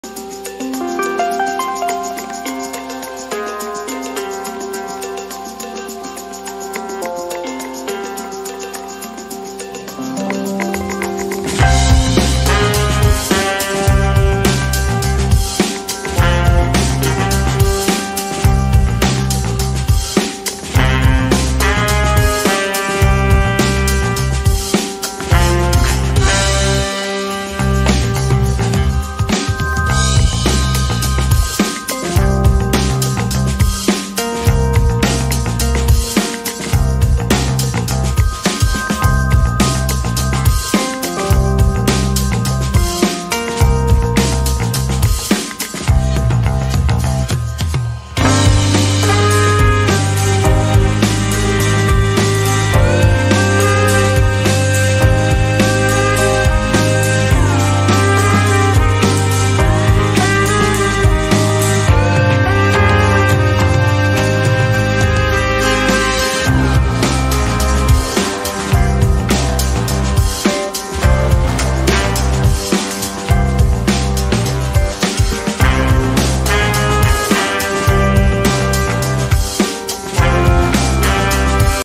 Aston Villa Puncaki Klasemen Liga Champions
John McGinn menyatakan bahwa ambisi Aston Villa di Liga Champions tidak mengenal batas setelah timnya mengalahkan Bologna 2-0. Hasil pertandingan yang berlangsung Rabu (23/1) dini hari WIB itu membawa Villa memuncaki klasemen.

Aston Villa terakhir kali tampil di level tertinggi sepak bola Eropa pada tahun 1983, namun transformasi yang dilakukan Unai Emery kepada  klub raksasa yang terlelap ini tak terbendung. Mereka menjadi satu-satunya tim yang meraih tiga kemenangan dari tiga pertandingan Liga Champions sejauh ini.

Gol pertama Villa tercipta melalui tendangan bebas McGinn yang meluncur ke sudut jauh di babak kedua. Jhon Duran, yang jarang dimainkan, menambah keunggulan dengan gol kedua.

Reporter: Beny Mudesta
Video Editor: Rifqi Rahmandika